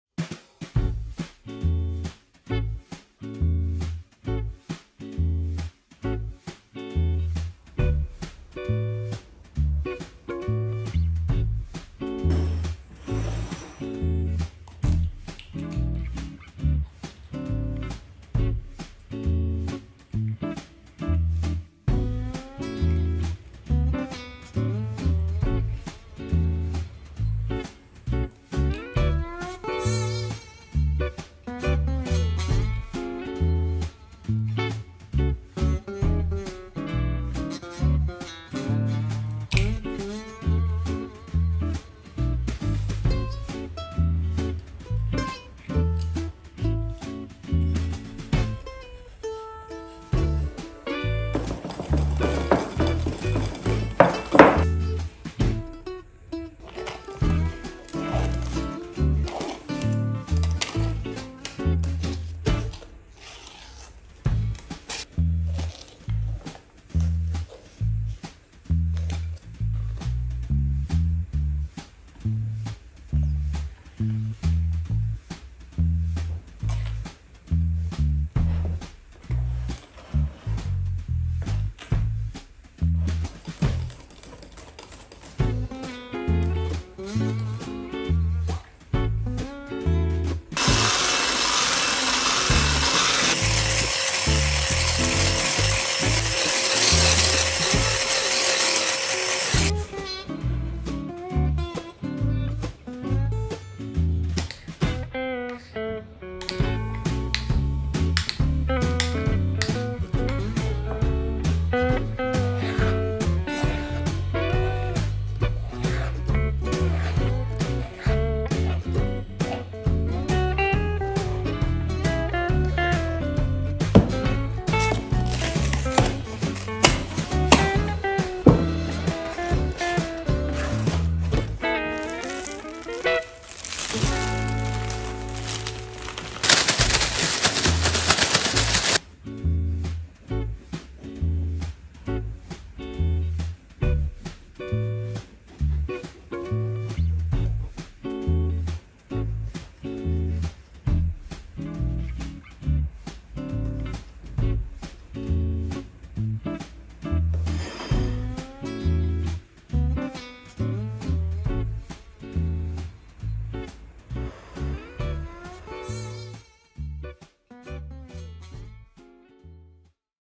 The easiest Italian Christmas dessert idea
Since I was young I remember that this dessert couldn’t miss on my table on Christmas! The recipe is super simple! I hope you will enjoy it!

For this recipe you need:

🌸4 eggs
🌸6 tbs of sugar
🌸500g Mascarpone
🌸1 Pandoro
🌸icing sugar